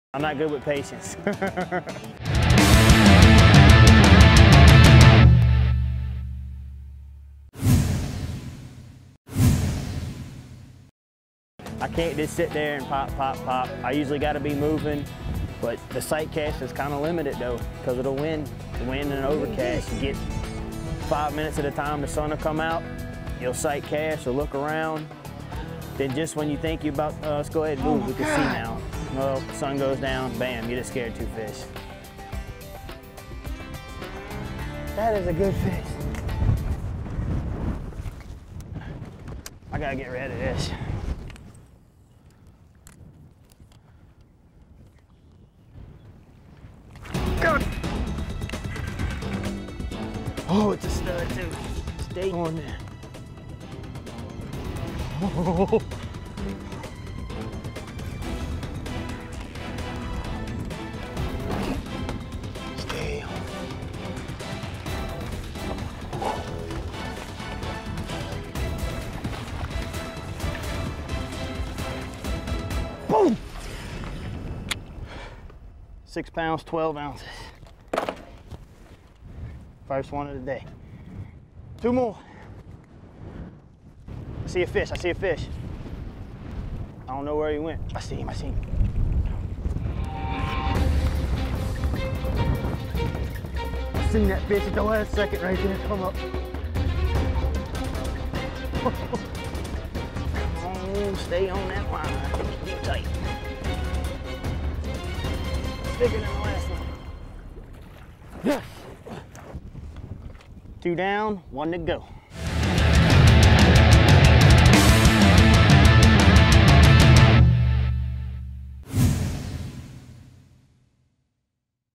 Elite Pro Bo Favre BACK TO BACK 2 CAST 2 FISH!
Pros from the tour compete head to head, no draw partners, no team mates. One angler per boat. Whether competing or sharing tips and tricks these guys are good, they are real good!